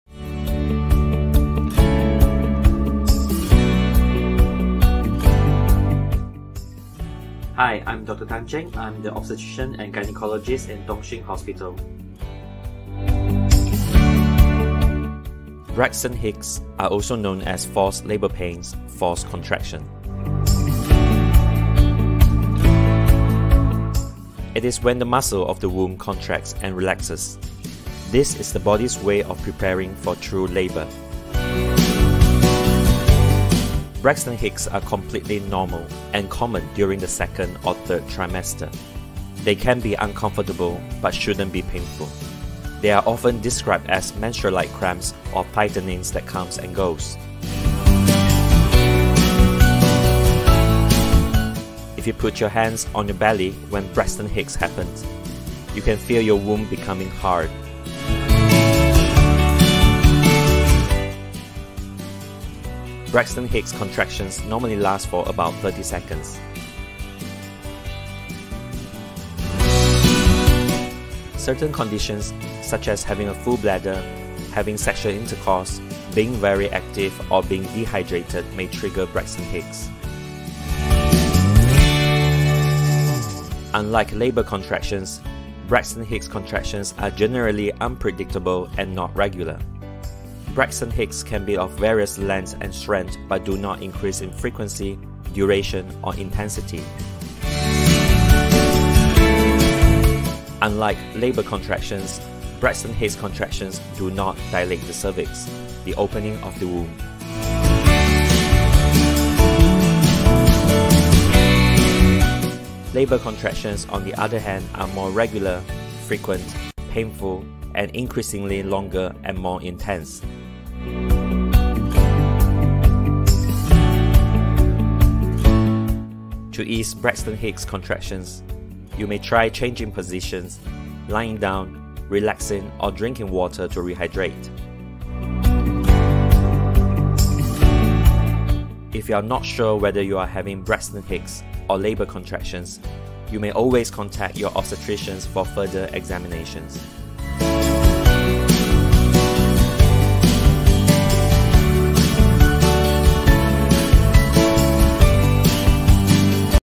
Braxton Hicks Contractions
Braxton Hicks are also known as false labour pains / false contraction. This the body’s way of preparing for true labour.